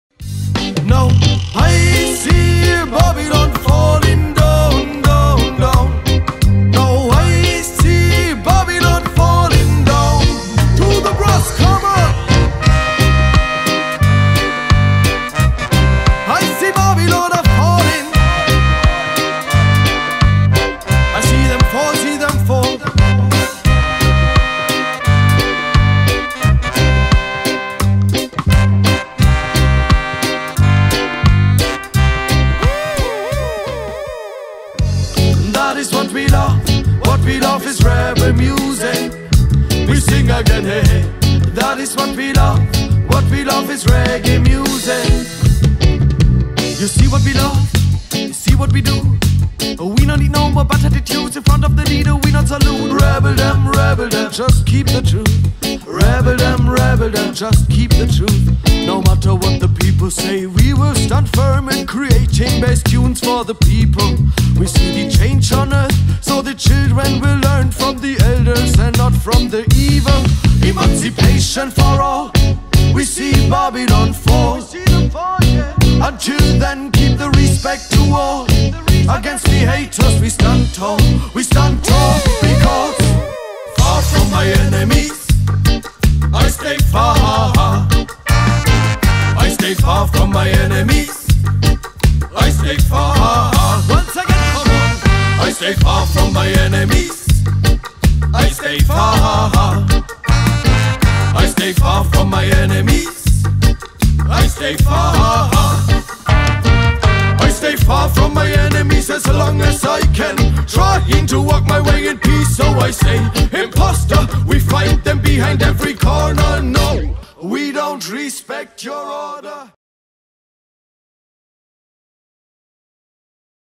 Headphonemusic Trailer @ 10. Sunshine Reggae Festival 2020
We can't wait Headphonemusic too next June on the stage of ROESCHWOOG in Camping Plage du Staedly ! More & More Beautiful People coming everywhere !!! See you there ... 👍☀️
See you there ... 👍🔥❤️
SO PROUD ! FIRST REGGAE FESTIVAL IN ALSACE SINCE 2011 !